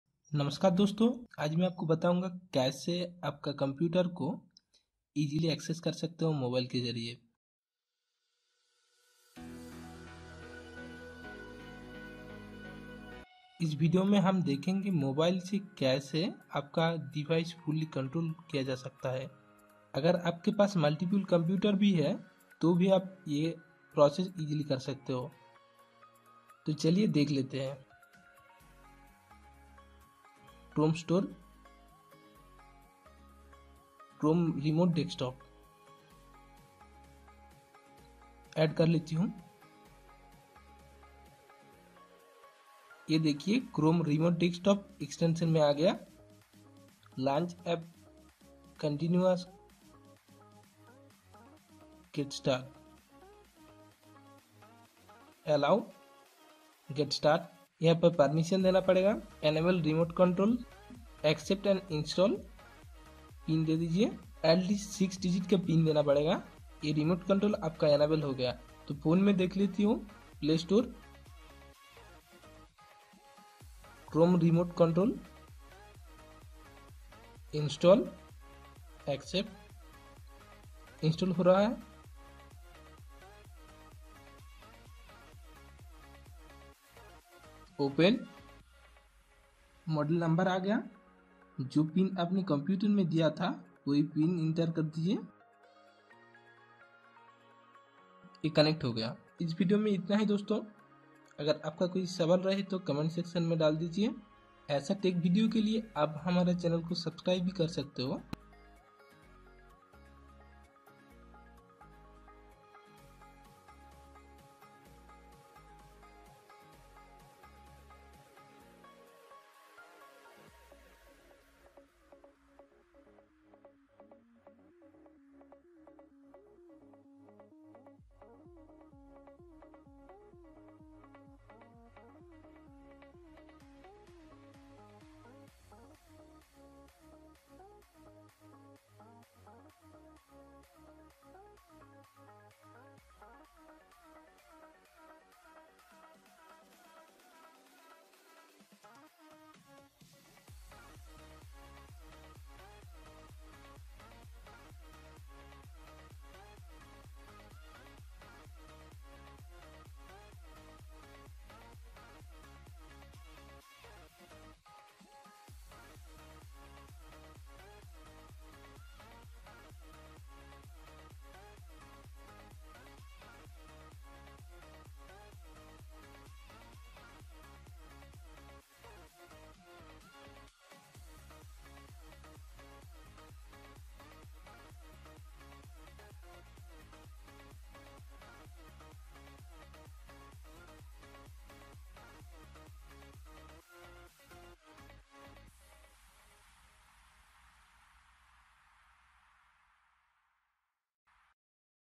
How to remotely control your PC from your Phone Anytime From Anywhere Anytime(New)
How to free remotely access your PC/Desktop connection from your Phone Anytime From Anywhere Anytime New